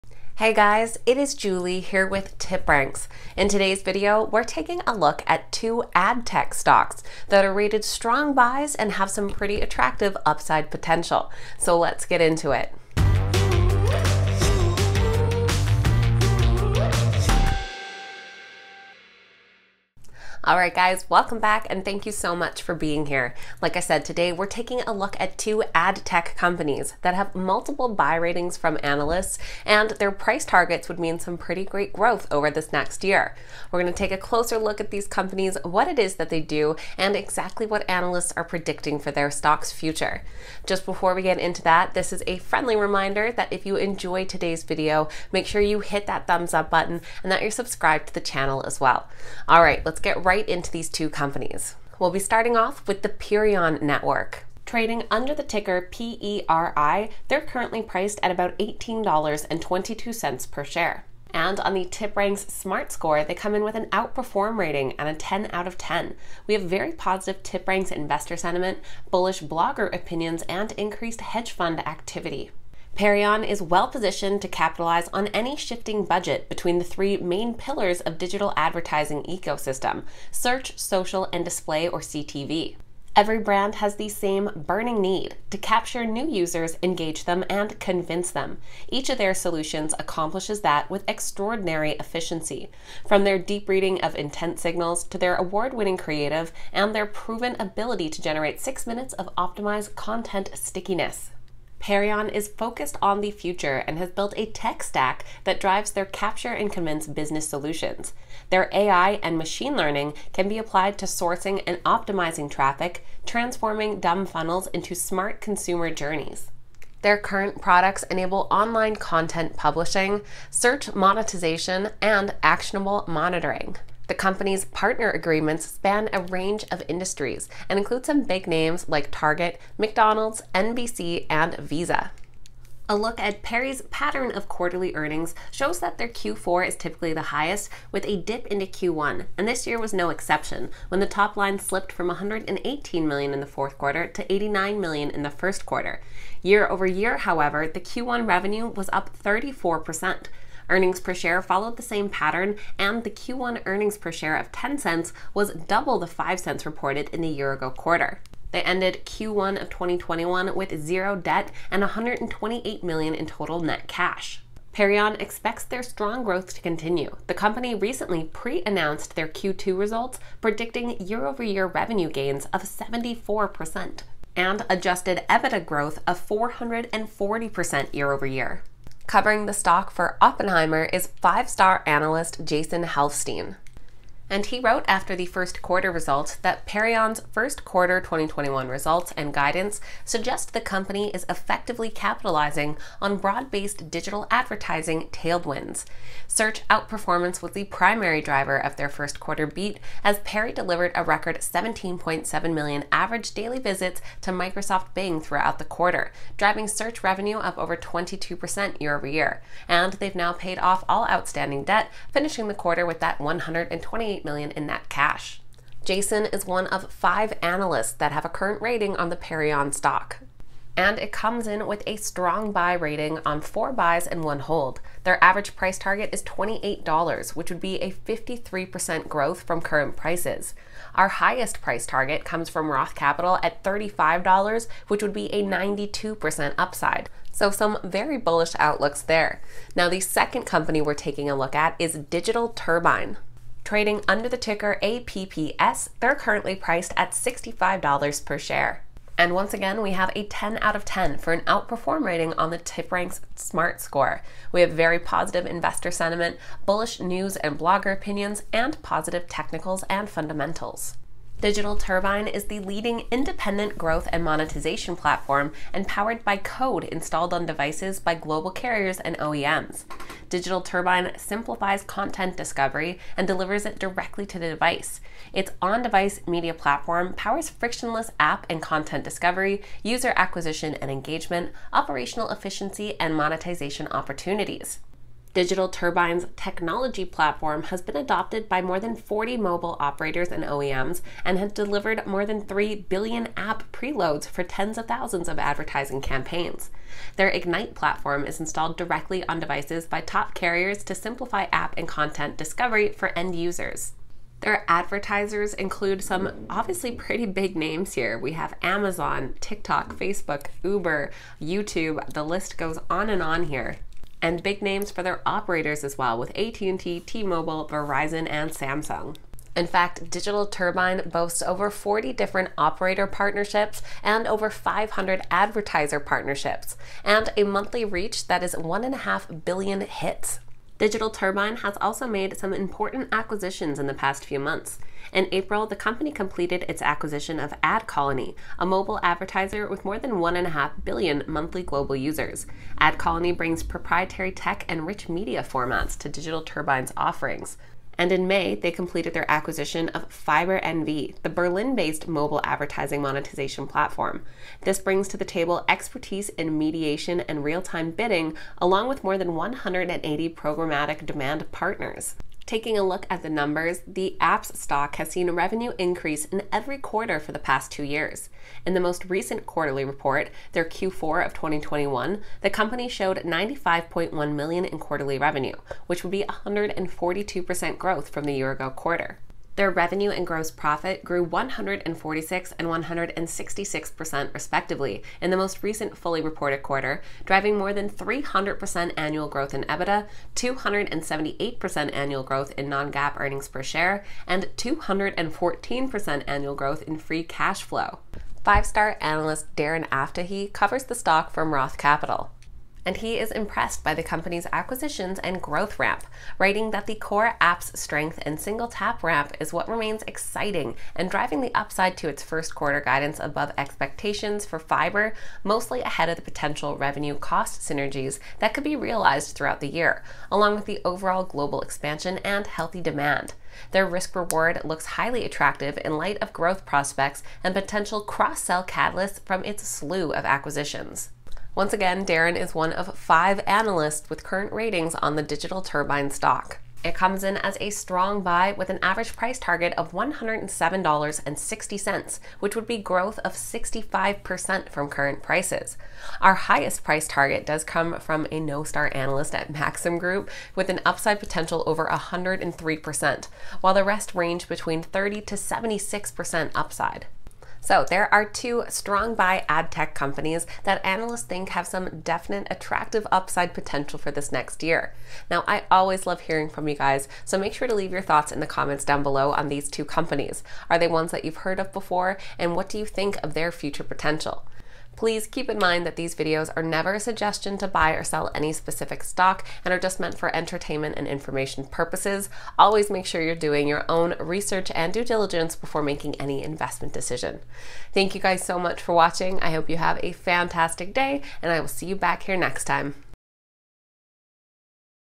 2 ‘Strong Buy’ Ad Tech Stocks Set to Spike by at Least 50%!!
Ad Tech requires specialized companies, that know the terrain of their environment and are ready to thrive in it. And that, of course, will bring opportunities for investors. Wall Street’s analysts are always busy, researching and publishing their takes on the market, and lately two 5-star analysts have tapped a couple of ad tech companies as winners in the near future. We’ve used the TipRanks platform to look up the details on these stocks; both are rated as Strong Buys and offer plenty of upside potential in the year ahead. Let’s take a closer look!

00:00 Intro
00:52 PERI
03:59 APPS

Perion, is a provider of engagement and monetization apps for web and mobile digital businesses. The company’s reach stretches across the developed world, with offices in, among other places, New York and DC, Chicago, LA and San Fran, Paris, Hamburg, Milan, and the Middle East. Perion’s original product, an email platform, shut down last year; current products enable online content publishing, search monetization, and actionable monitoring. The company’s partner agreements span a range of sectors, and include McDonald’s, Target, NBC, and Visa.

Digital Turbine, is an ad tech software company that develops apps and code to enable growth and monetization in the digital ad field. The company’s software makes it possible for mobile operators to more accurately target content and ads, putting the revenue-generating material directly to the right recipient. The company’s partners include net giants like Amazon (AMZN) and Facebook (FB), as well as LinkedIn, Instagram, Wish, and HBO.